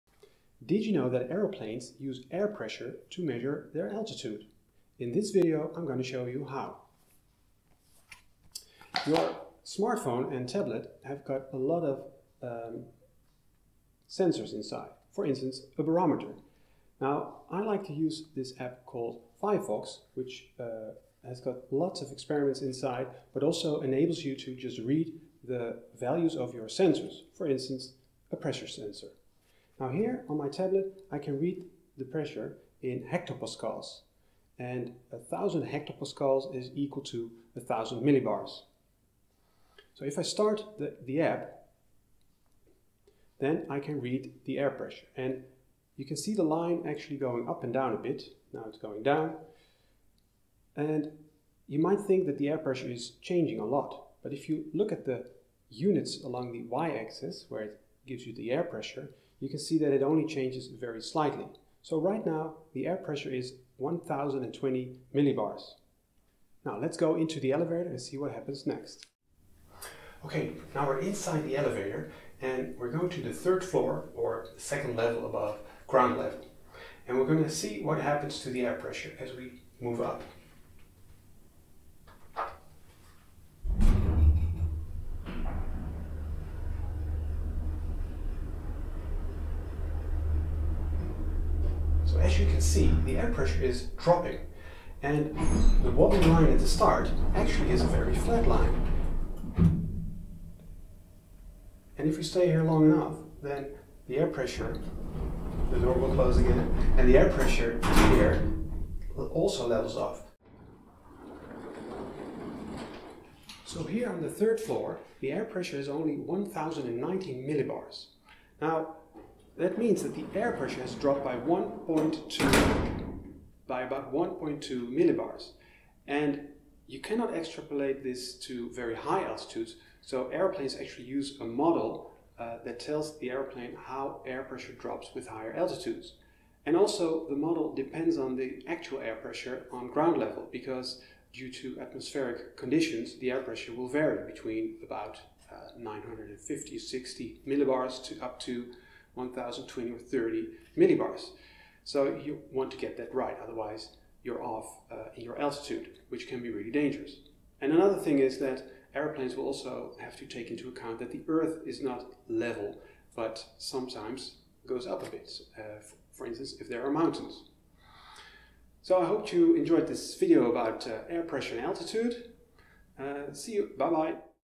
Measuring Altitude with Air Pressure
You can use your tablet's or smart phone's barometer sensor to measure altitude. That's the principle aeroplanes rely on to measure their altitude. Here I demonstrate this principle going from the ground floor up to the second floor.